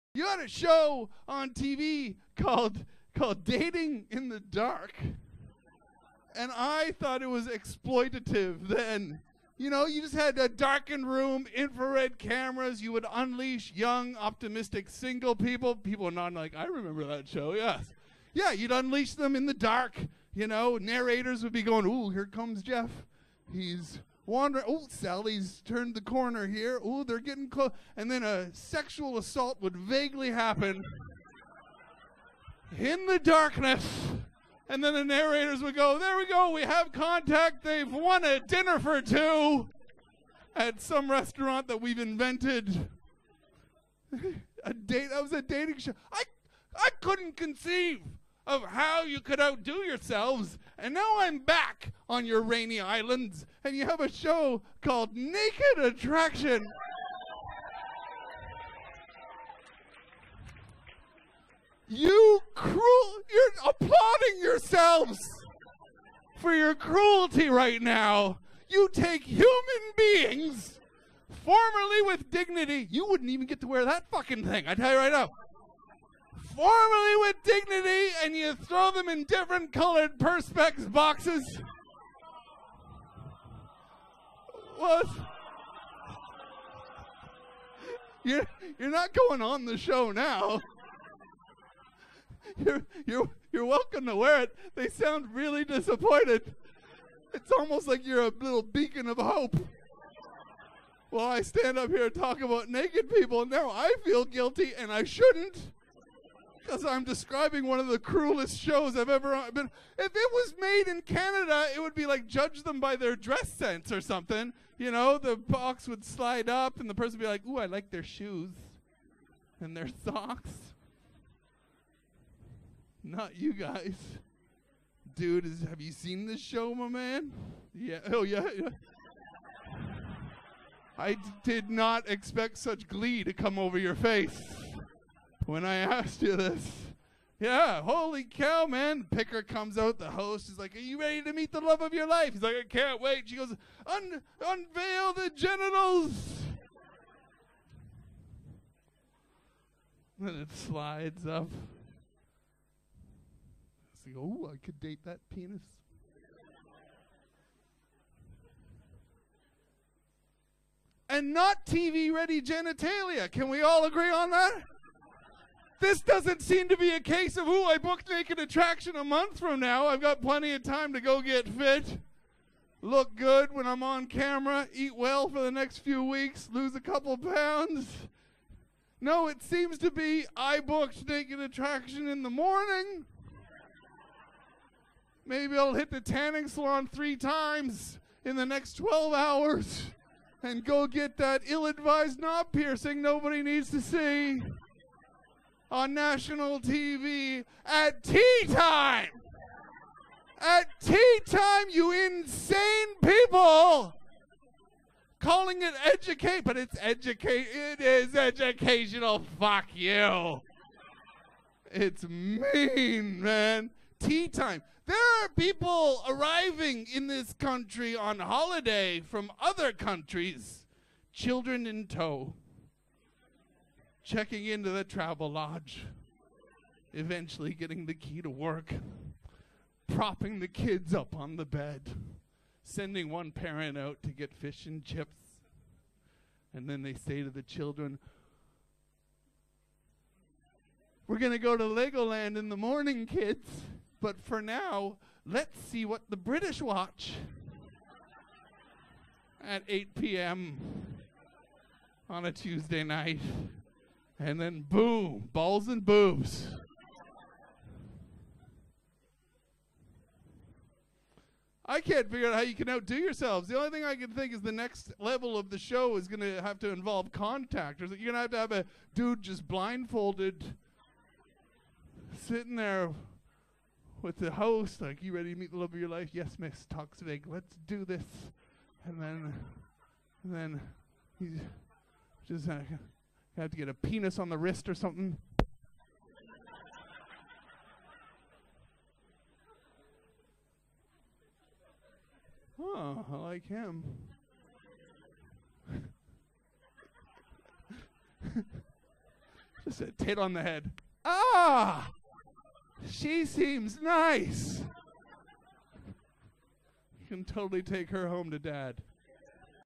What's Your Favorite Game Show?
Have You Ever Seen The TV Show Naked Attraction?
Indeed the UK has some interesting game shows under the guise of 'Education'.